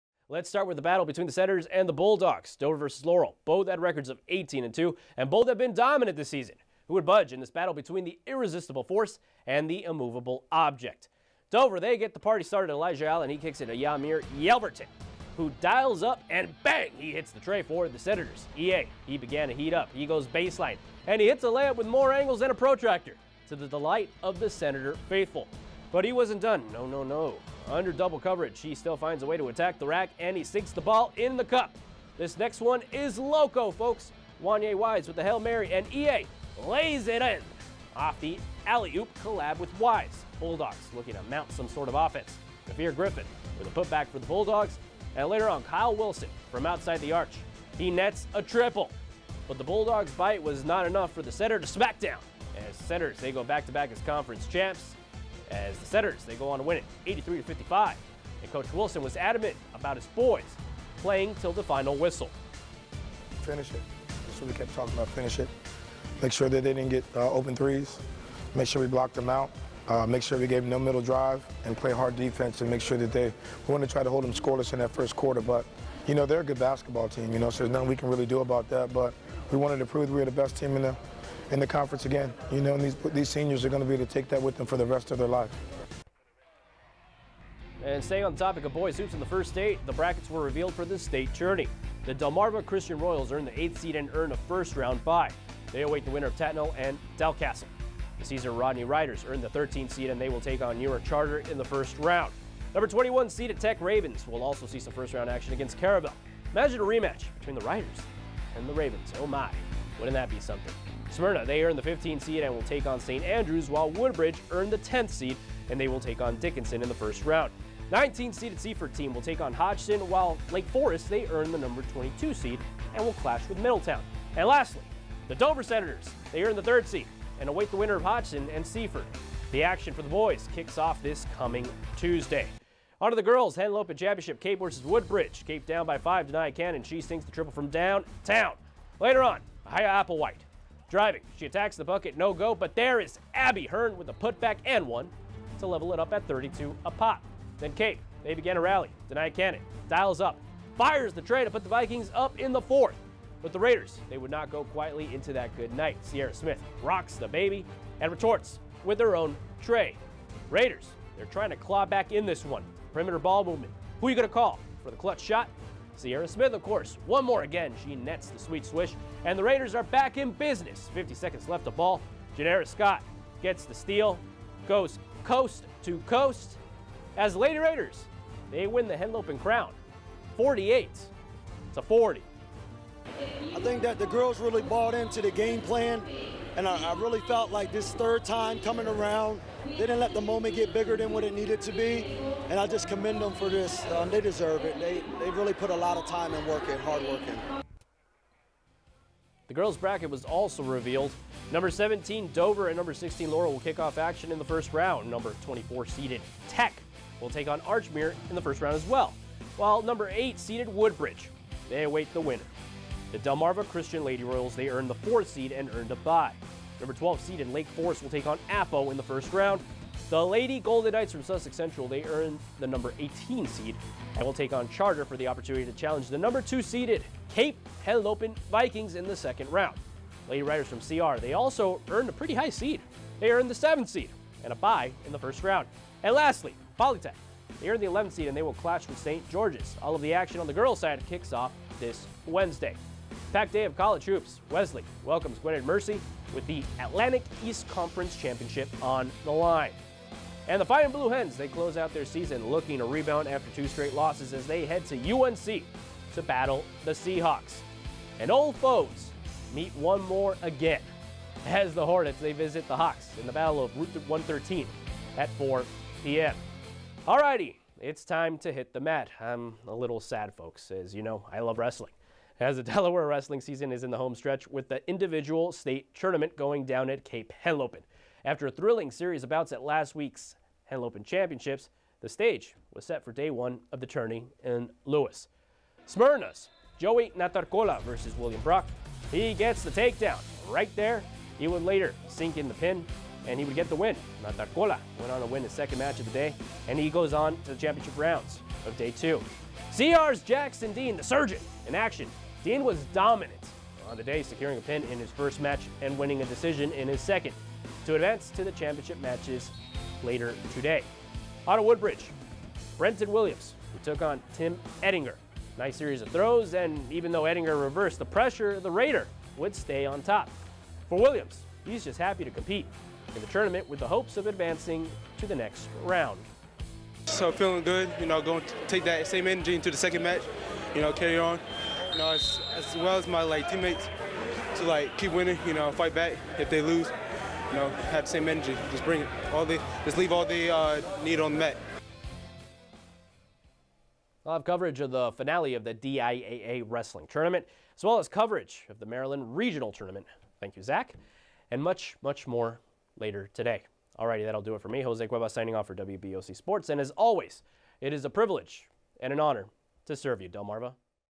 Dover defeats Laurel, Woodbridge defeats Cape Henlopen, a preview of the DIAA Basketball Tournament
Relive the thrilling Henlopen Championship games between Laurel High School and Dover High School and Woodbridge Raiders Athletics and Cape Henlopen High School

Also coverage of Day 1 of the DIAA Wrestling State Championships with footage of Smyrna Wrestling's Joey Natarcola and Caesar Rodney "Riders" Alumni Wrestling Page's Jackson Dean

and a rundown of the Delaware girls and boys basketball tournament